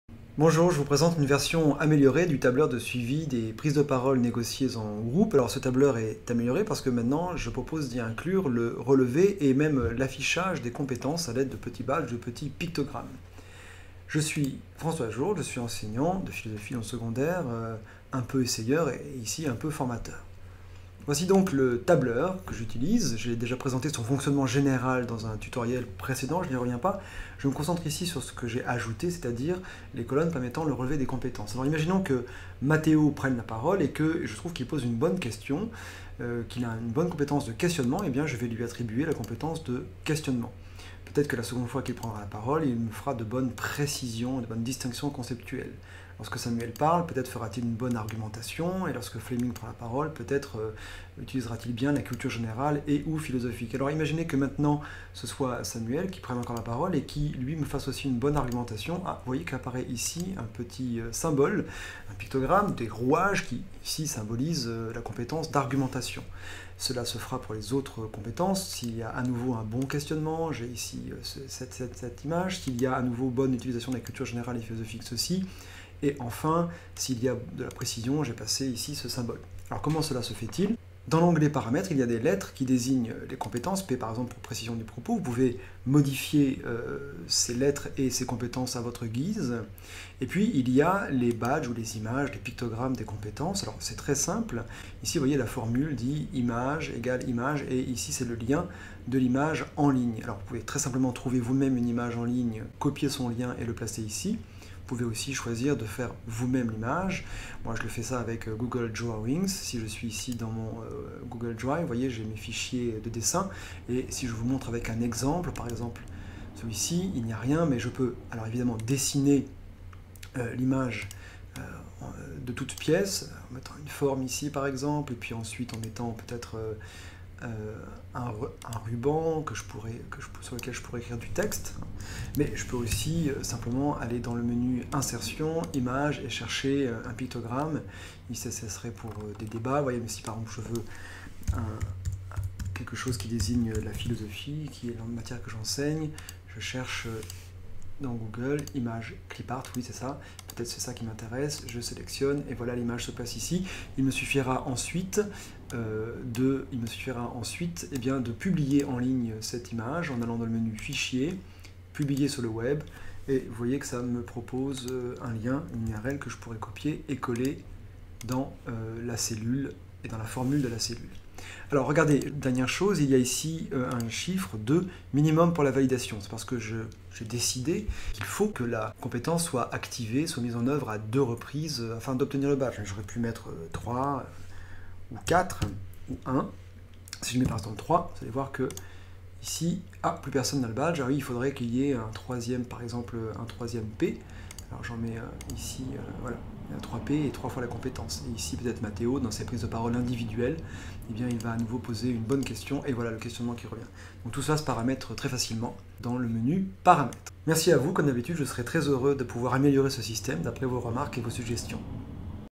Tableur de suivi de prises de paroles négociées & compétences
Explication du fonctionnement du tableur de suivi de prises de paroles négociées en groupes incluant le lemevé des compétences.